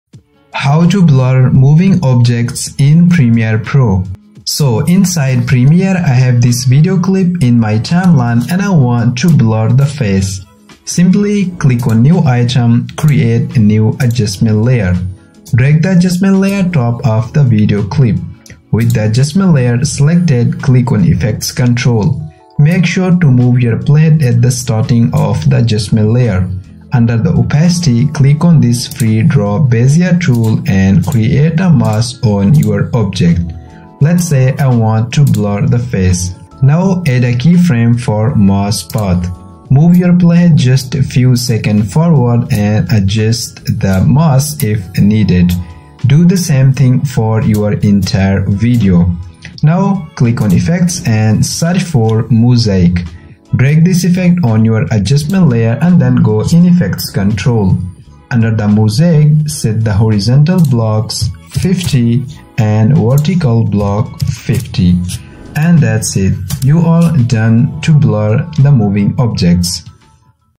How To Blur Moving Objects In Premiere Pro | Motion Tracking Blur Tutorial
Learn how to blur moving objects in Adobe Premiere Pro with this step-by-step tutorial! Whether you're looking to censor sensitive information, hide faces, or add a creative touch, this video covers how to apply a dynamic blur that follows any moving object. This is perfect for vloggers, filmmakers, and editors who need to add a professional blur effect with smooth motion tracking.